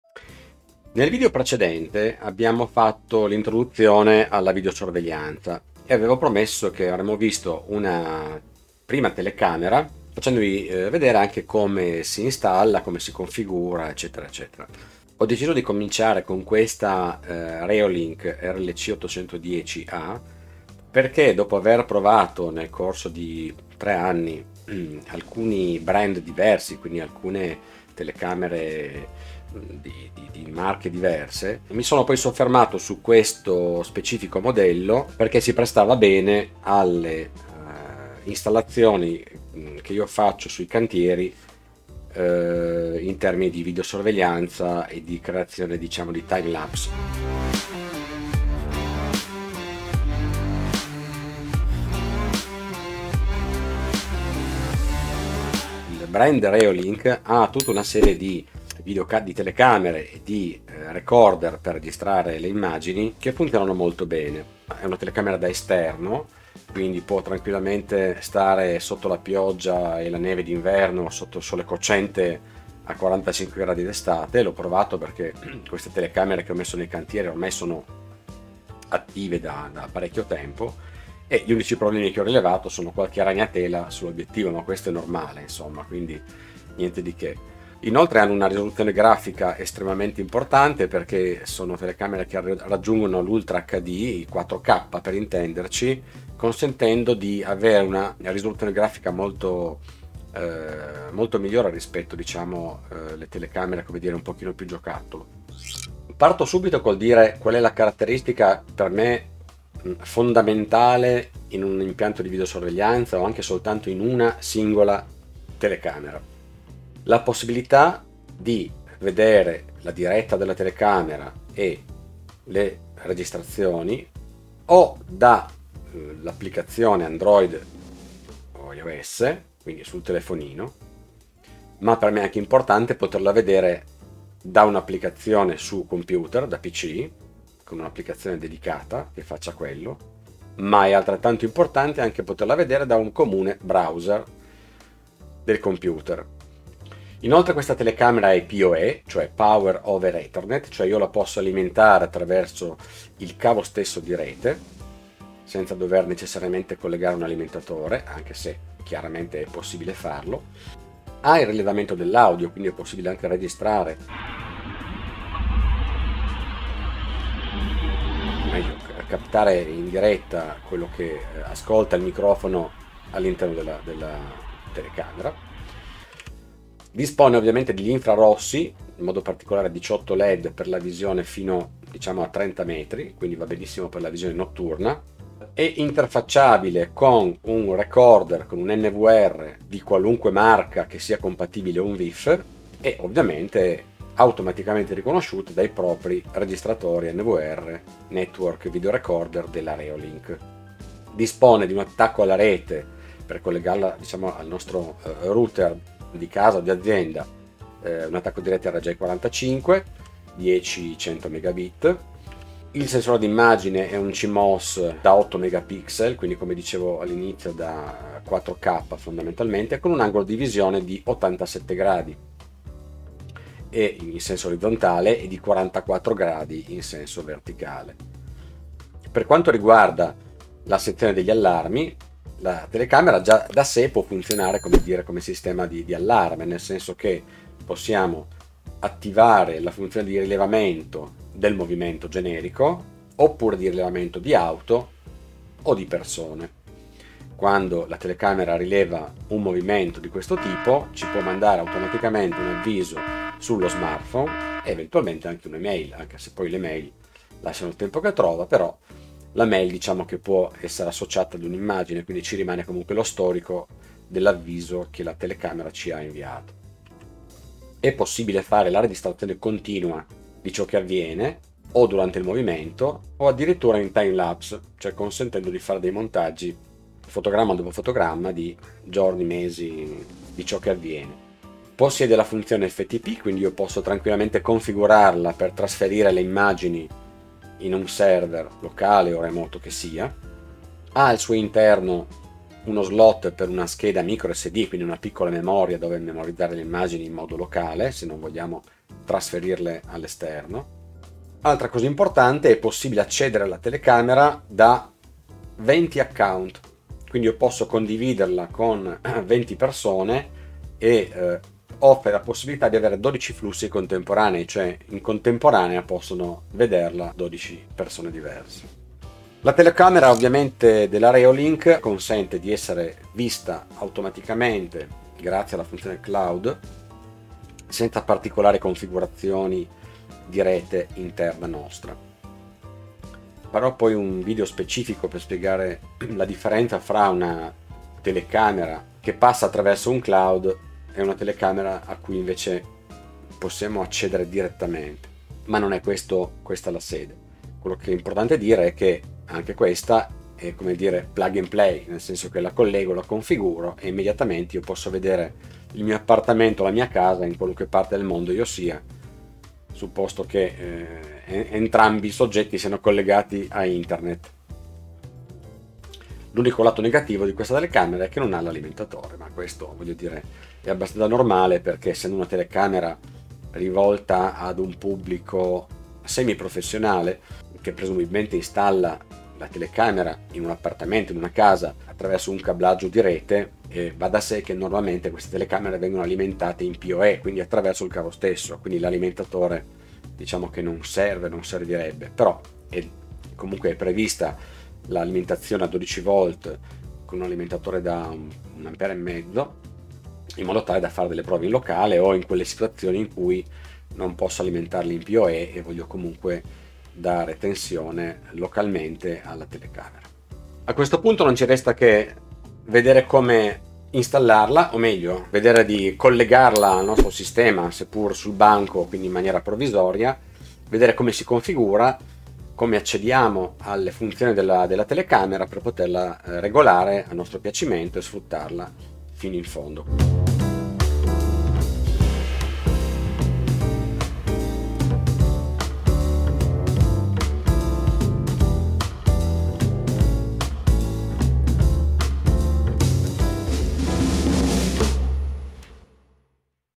Telecamera REOLINK 810A - Perché ho scelto questa
Nel mio lavoro utilizzo le telecamere per videosorveglianza reolink ( @ReolinkCameras ) perché dopo diverse prove con altri brand sono giunto alla conclusione che erano (e lo sono almeno sino ad oggi 2022) quelle più confacenti alle mie necessità e con un rapporto qualità / prezzo decisamente adeguato.
L'assistenza è a dir poco eccellente, considerando che rispondono dall'altro capo del mondo e il loro sito è ricchissimo di documentazione e FAQ
Inoltre, anche se non ne parlo in questo video, ho integrato queste telecamere anche con il sistema domotico homeassistant, oggetto di un successivo video.

Questa non è una recensione classica ma una breve introduzione alle caratteristiche che mi hanno portato a sceglierla per l'impiego ad un mio progetto-lavoro relativo a delle riprese "timelapse" nei cantieri dove le condizioni non sono per nulla favorevoli.
Nel prossimo video vedremo la sua accensione e prima attivazione in modo tale che anche i meno esperti (mi auguro) ne possano trarre vantaggio.

🛒 La telecamera in questione è la Reolink modello RLC 810A (stile "Bullet") che potete trovare a questo
Altri modelli:
Modello "Dome" 8Mpx RLC-820A
Modello "Dome" 5Mpx RLC-520A
Modello "Dome" con zoom 3X 8Mpx RLC-822A

CAPITOLI :
00:01 Start e introduzione
00:20 Perchè l'ho scelta
01:04 Telecamera da esterno
01:28 Elevata qualità nella risoluzione
01:50 La completezza dei supporti Software
02:27 Alimentazione attraverso POE
02:41 Registrazione dell'audio
02:56 Infrarossi potenti
03:07 Compatibile ONVIF
03:21 Collegamento LAN
03:33 Sensore di immagine e angolo di visione
03:49 Allarmi impostabili
04:27 Registrazione continua o su allarmi o timelapse
04:41 Funziona FTP
04:51 Slot microSD
05:01 Multiutenza
05:22 Funzione Cloud inclusa
06:07 La pecca, manca l'alimentatore
06:55 L'appuntanento al prossimo video

Buona Video Sorveglianza